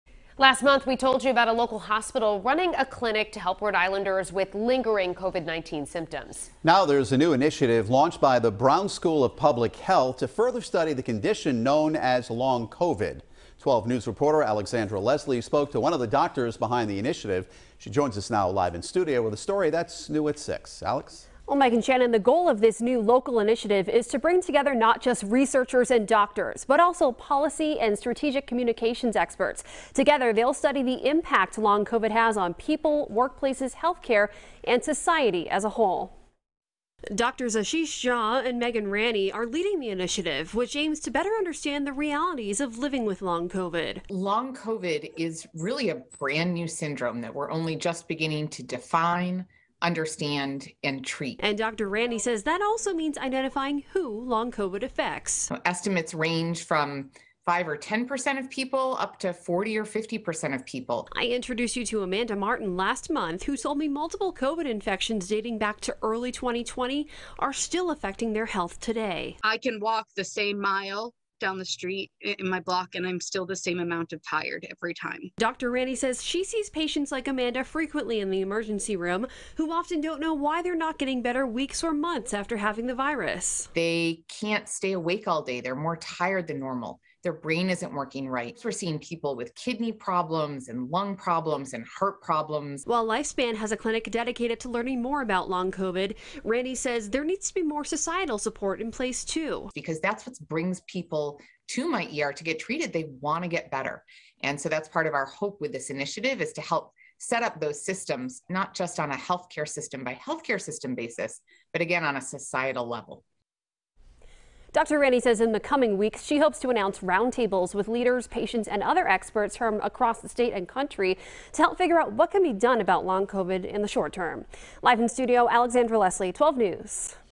Brown University launches initiative to research effects of long COVID
Doctors and pandemic experts in Rhode Island are expanding on a recent initiative by the National Institutes of Health, which focuses on studying the clinical aspects of long COVID.